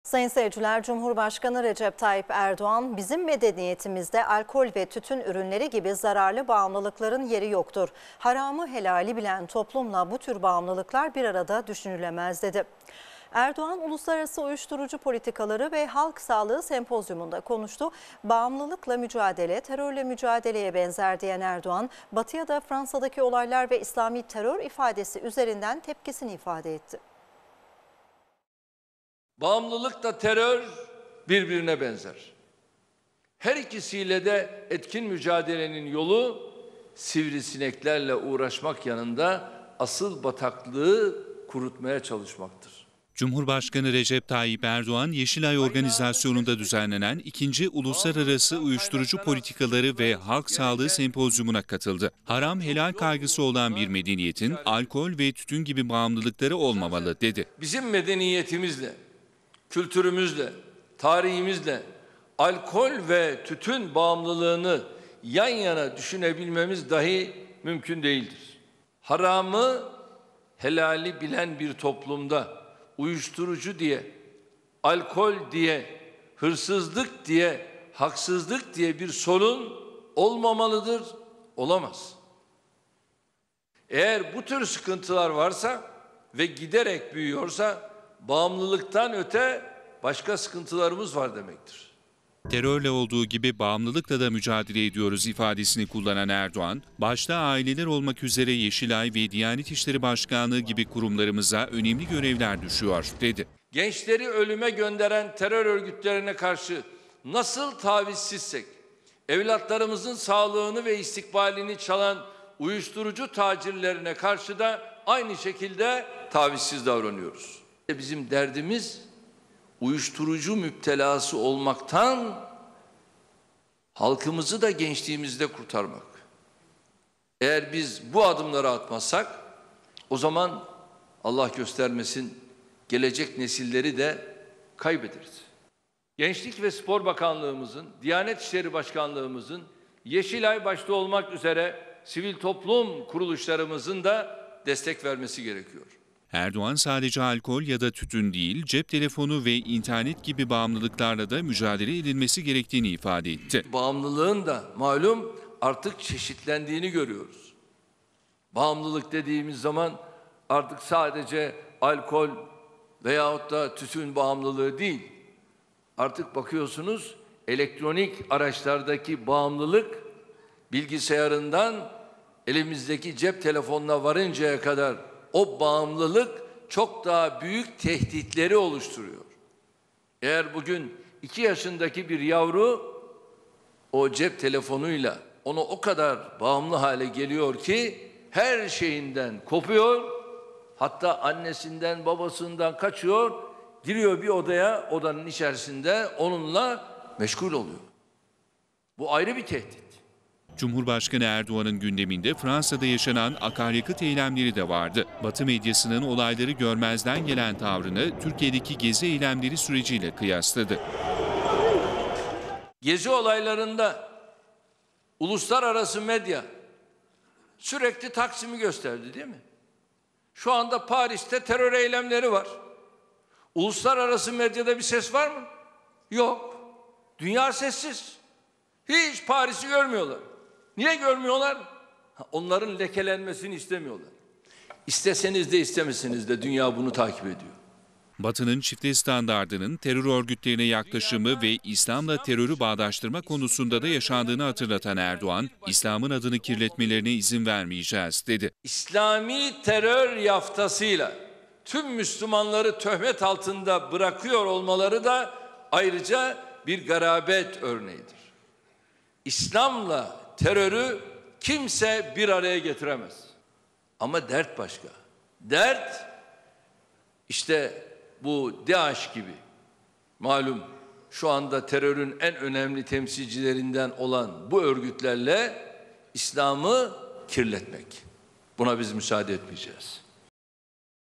Erdoğan’dan “Bağımlılık” Uyarısı
Cumhurbaşkanı Recep Tayyip Erdoğan, “Bizim medeniyetimizde, alkol ve tütün ürünleri gibi zararlı bağımlılıkların yeri yoktur. Haramı helali bilen toplumla bu tür bağımlılıklar, bir arada düşünülemez” dedi.  Erdoğan, “Uluslararası Uyuşturucu Politikaları ve Halk Sağlığı Sempozyumu”nda konuştu. “Bağımlılıkla mücadele, terörle mücadeleye benzer” diyen Erdoğan, batıya da Fransa’daki olaylar ve “İslami terör” ifadesi üzerinden tepkisini ifade etti.